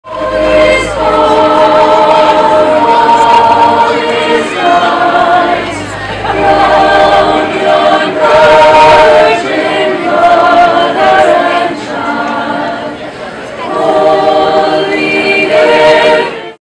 Jacksonville Christmas Carolers
Another beautiful night in our little town of Jacksonville. A special event including tree lighting and a local choir made things just about perfect!